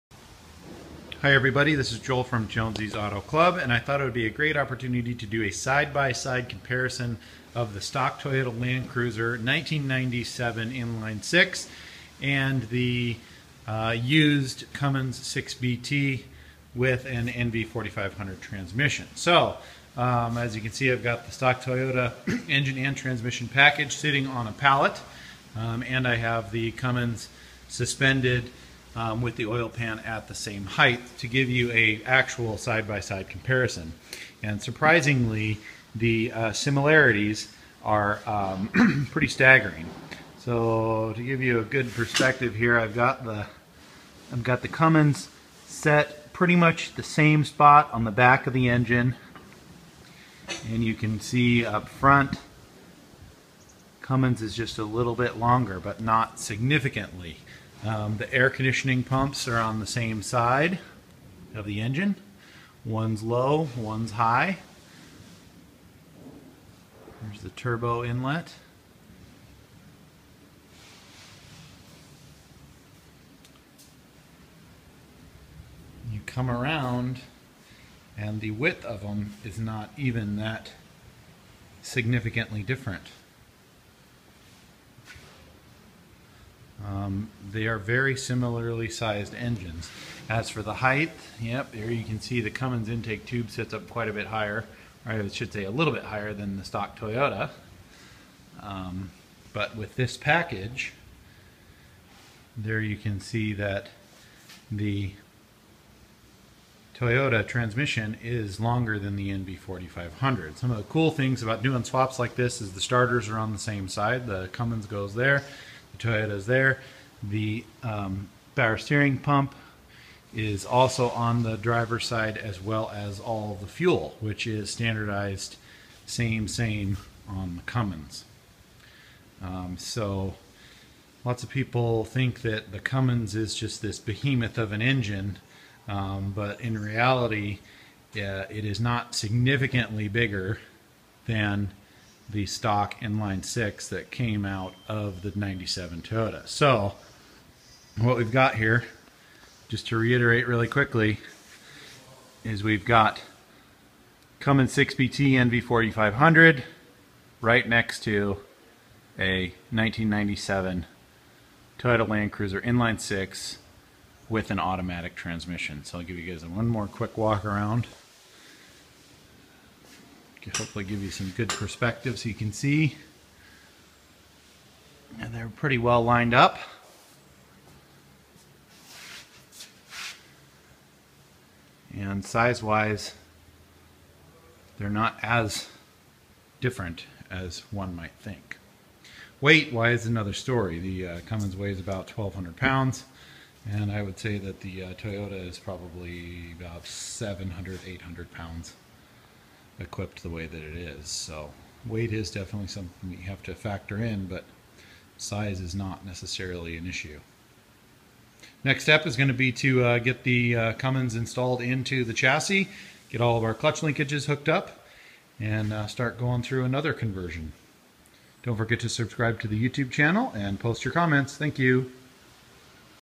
Cummins 6BT 5.9L vs Toyota Land Cruiser FZJ80 Stock 4.5L
In this video we do a side by side comparison of the Cummins 6BT Turbo Diesel we will be replacing the stock Toyota Land Cruiser FZJ80 4.5L gas engine with. In the video you can see both engines are of similar dimensions.

Doing a DIY diesel swap?  We’ve got you covered!

AmazonAssociate - We earn a small portion from qualifying purchases.